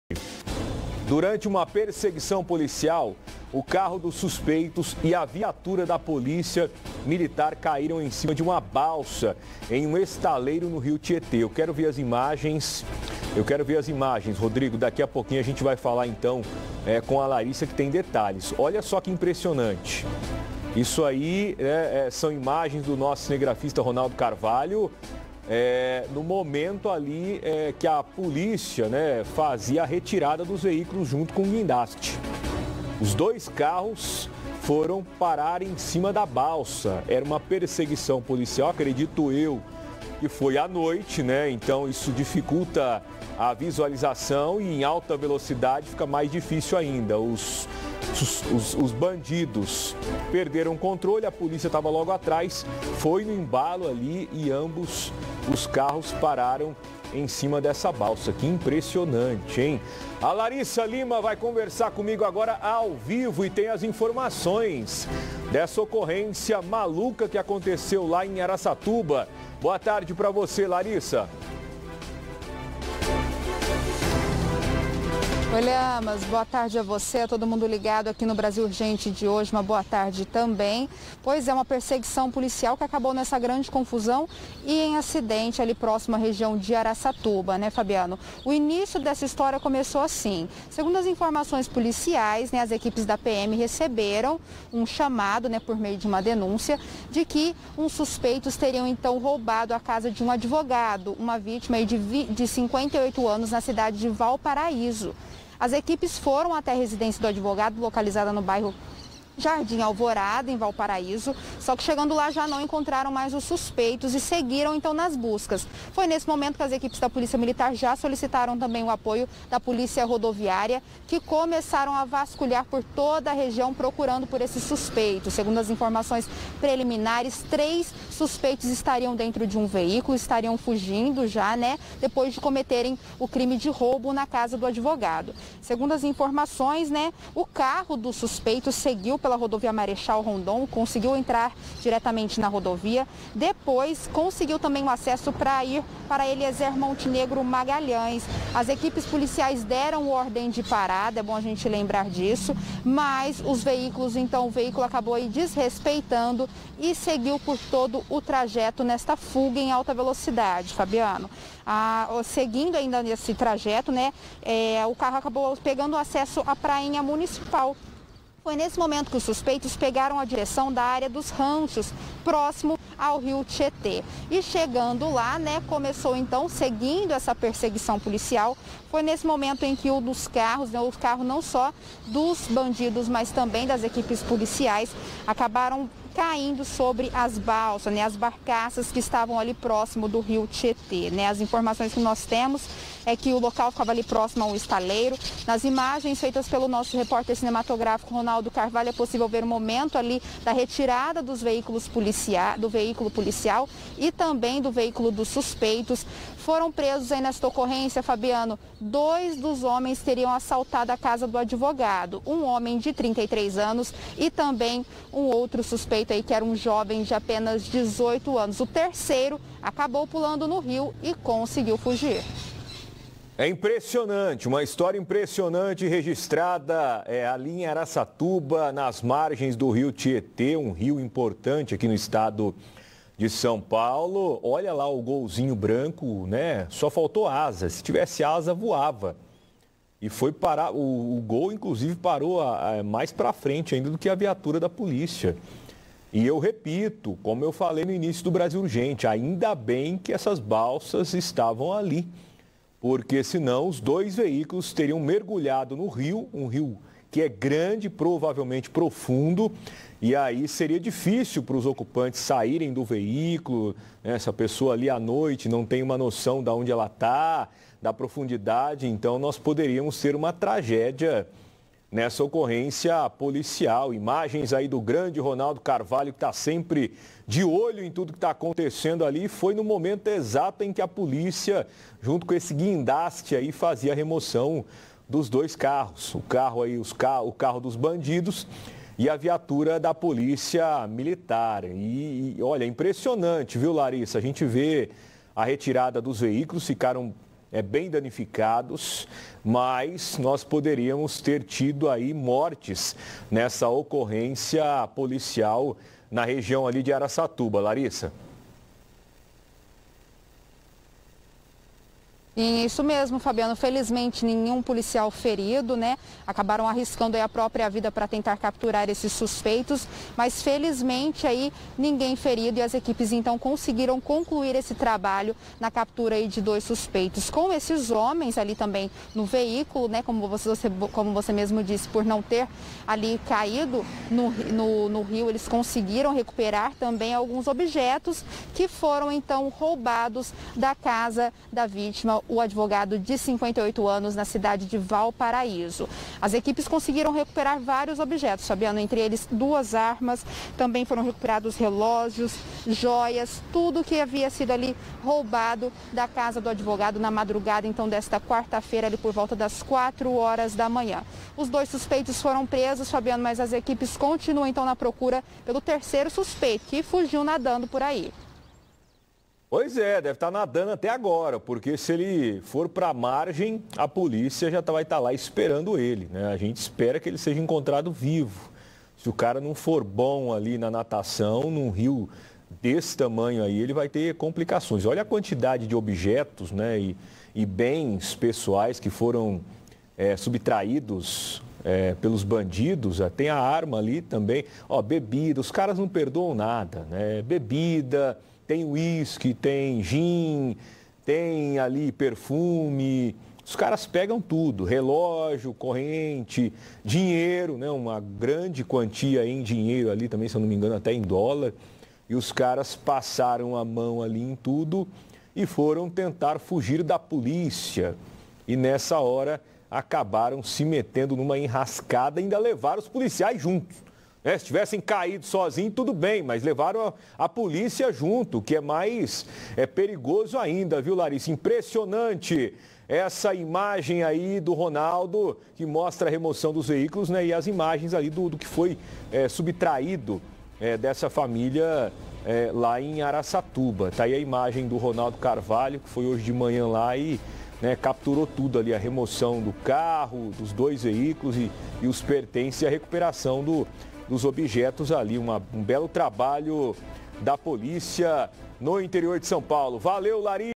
PERSEGUIÇÃO TERMINA EM ACIDENTE NO TIETÊ
Veículos caíram em balsa no momento em que ocorria uma perseguição policial por roubo, na região de Araçatuba.